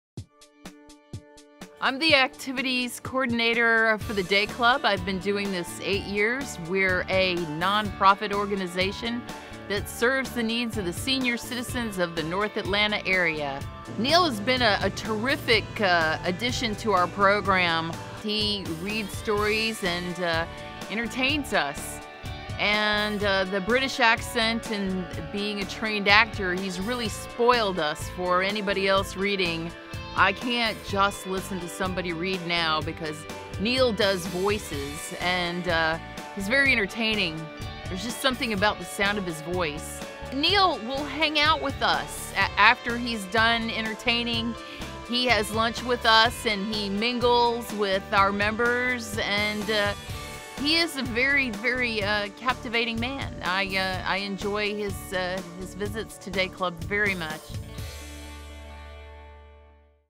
Activity Director talks to Neil
Susan King talks about expressive reader Neil Scott-Barbour